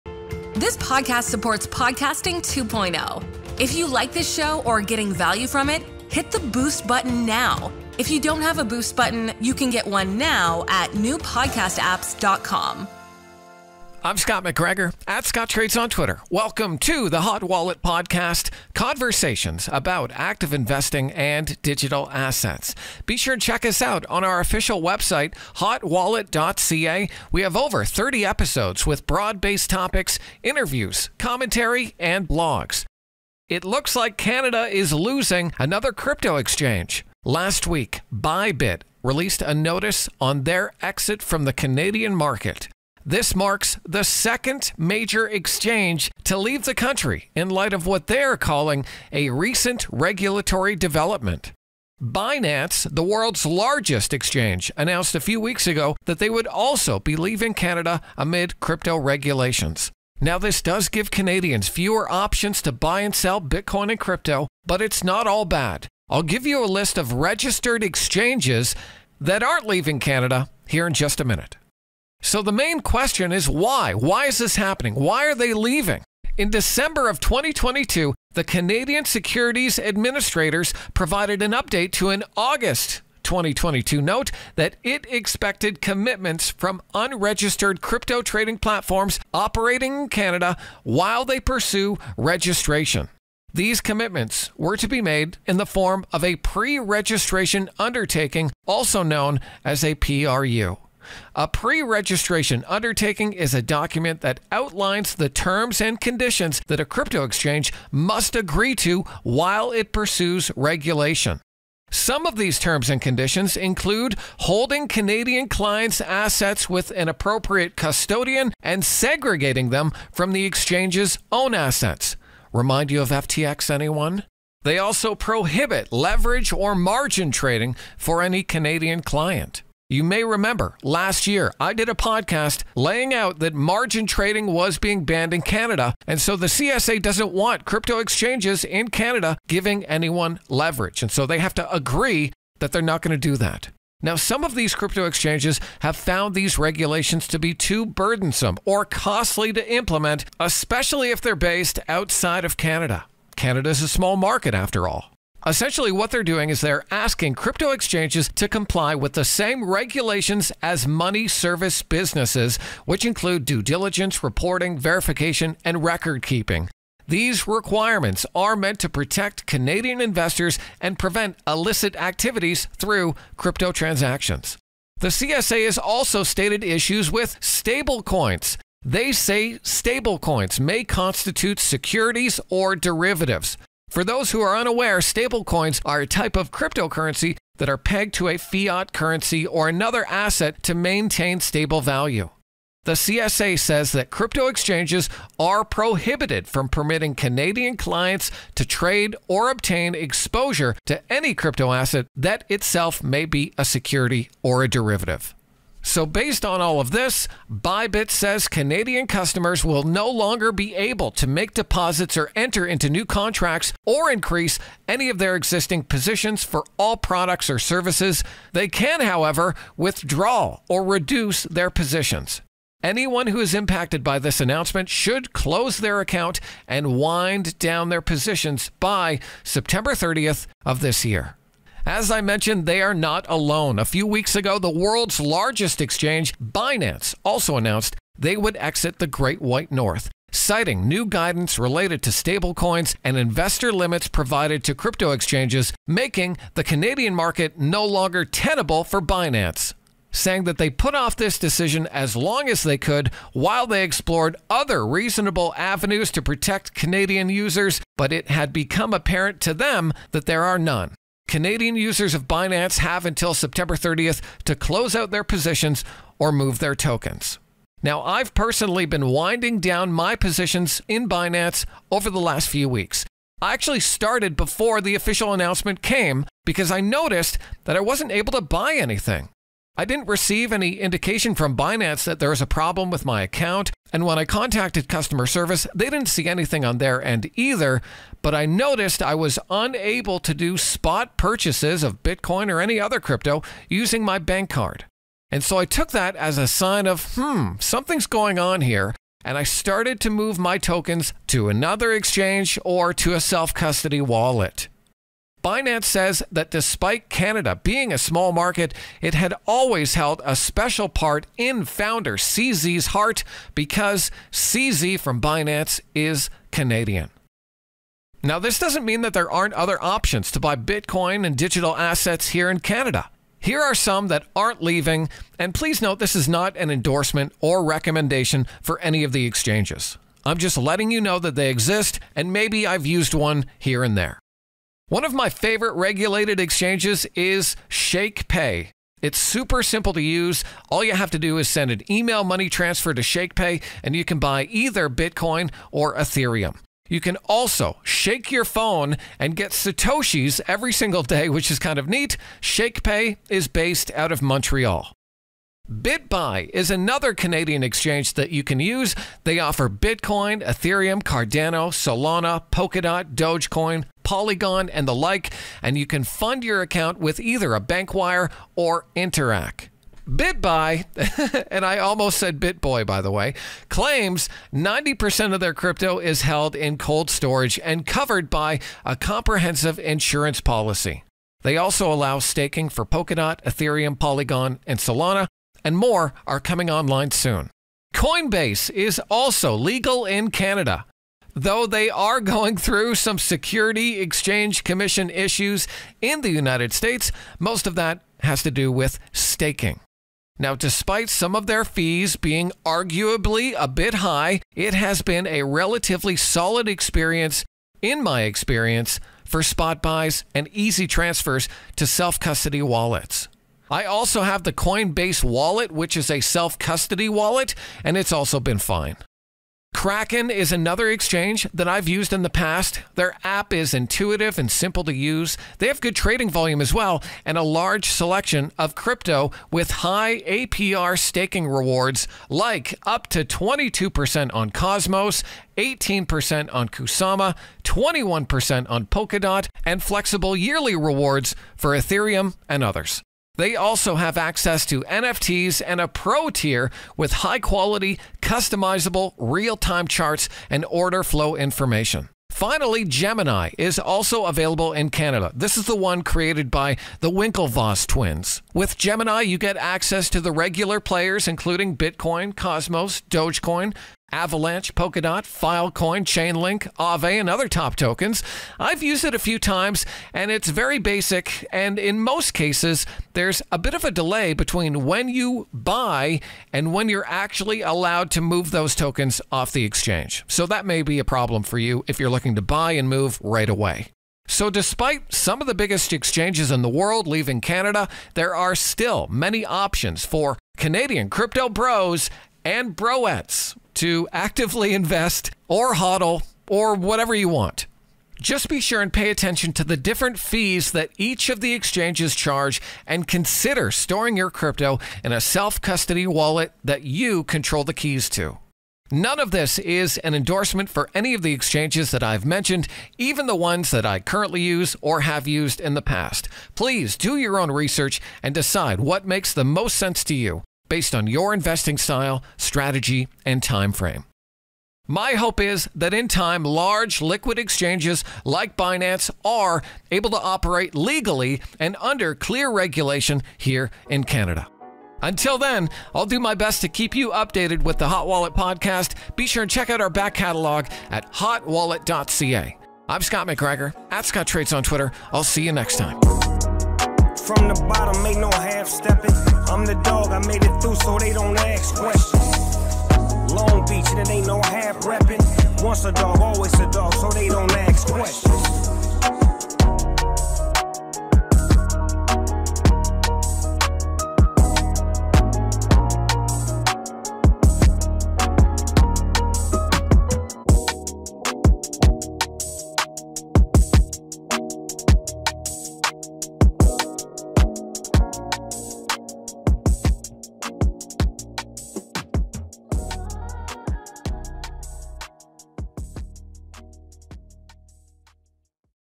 Canadians: Binance & BuyBit are Gone! Here's Your New Crypto Exchange Guide
Canada is facing a crypto crisis as two of the biggest players in the industry, Binance and BuyBit, pack their bags and say goodbye. In this episode of Hot Wallet, we'll explore the reasons behind their departure and what it means for you as a Canadian crypto investor. Scott will also give you some tips on how to find other platforms that suit your needs and preferences. Whether you're into Bitcoin, Ethereum, Polkadot, Solana, Cosmos, or any other digital asset, you don't want to miss this.